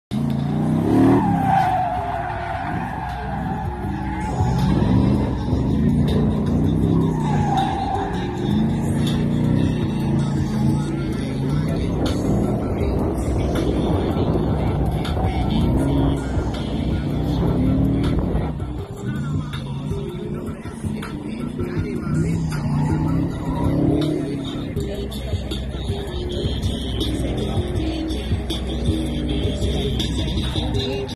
Drift Truck @ MB Drift's Sauce Team Tandem Event
Finally getting some seat time after confirming that crankcase pressure was no longer an issue!